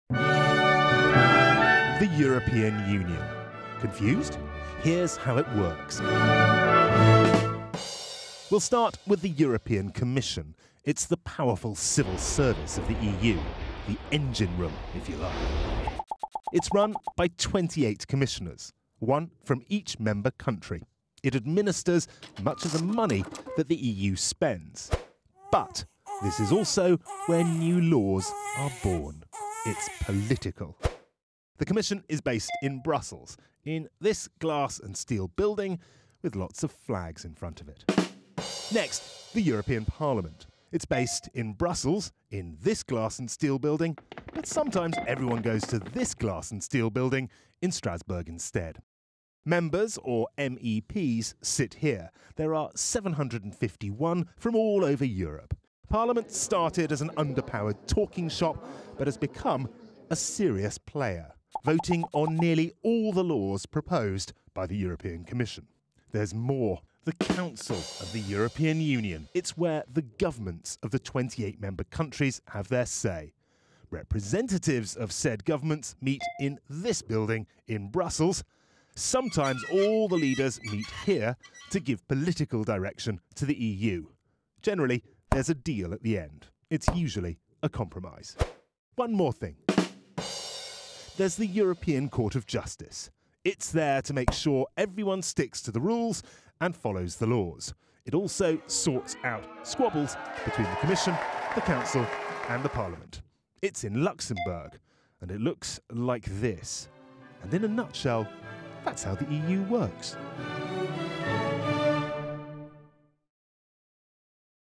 EU: All you need to know in under 2 minutes - BBC News
The BBC's Jonny Dymond explains how the European Union works in under two minutes.